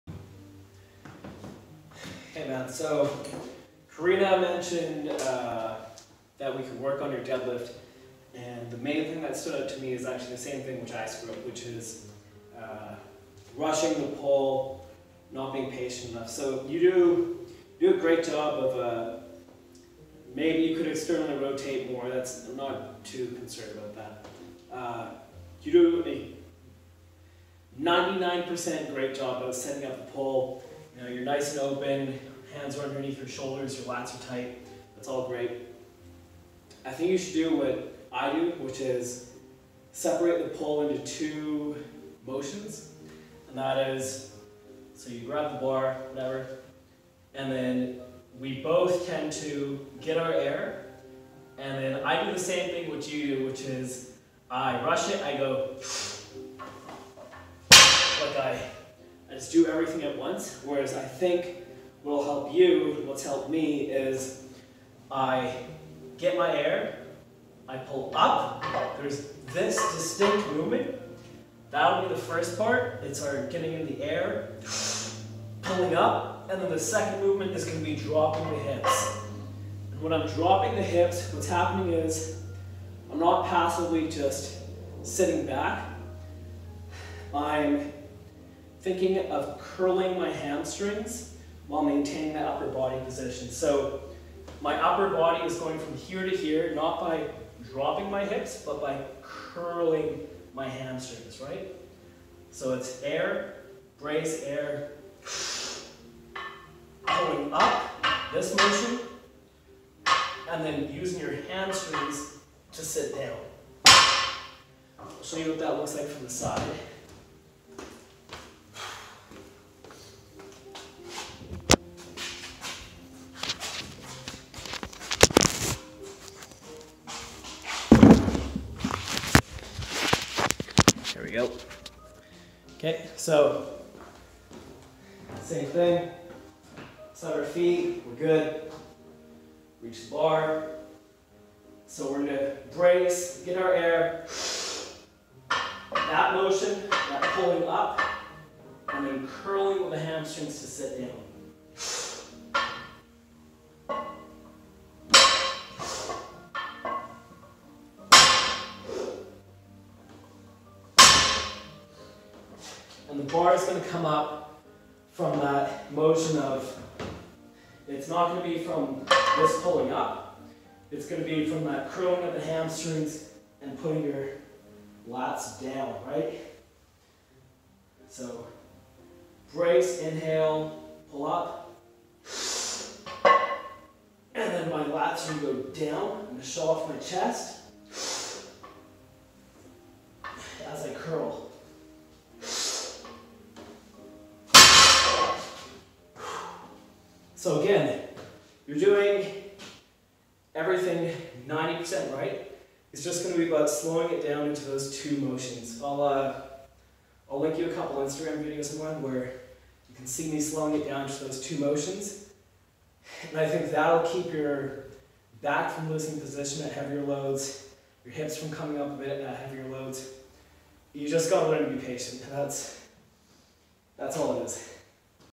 Patience Levering
Describing how to break the deadlift into two pieces:

1) Bracing/inhaling and pulling up/getting whip out of the bar
2) Drawing into tension using the hamstrings and depressing the lats/showing off the chest (while maintaining neutral spine)

Patience is key to not losing back and hip positioning. Draw into tension!